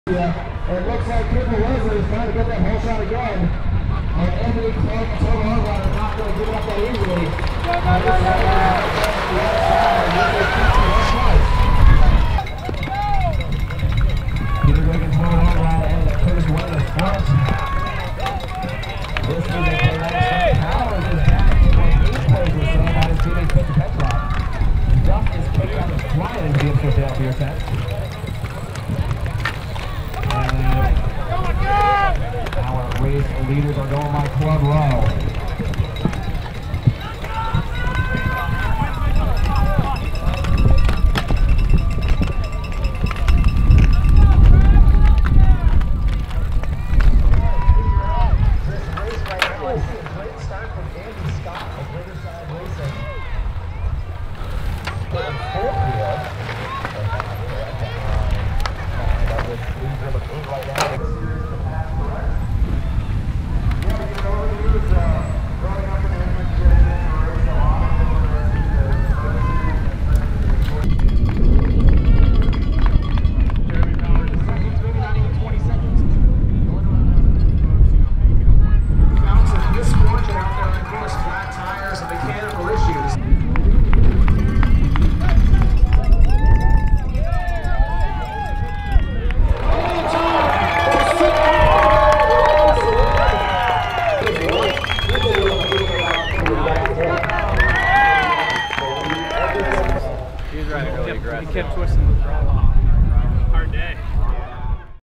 ADHDTV-HIGHLIGHTS: GP Gloucester Day 2 - Elite Men
Lots more highlights with POV GoPros and gimbal shots to come.

CRAFT Sportswear Gran Prix of Gloucester Presented by Ipswich Brewing, Vittoria Northeast 1 & 2
Gloucester, MA, Sat Oct 14 - Sun Oct 15, 2017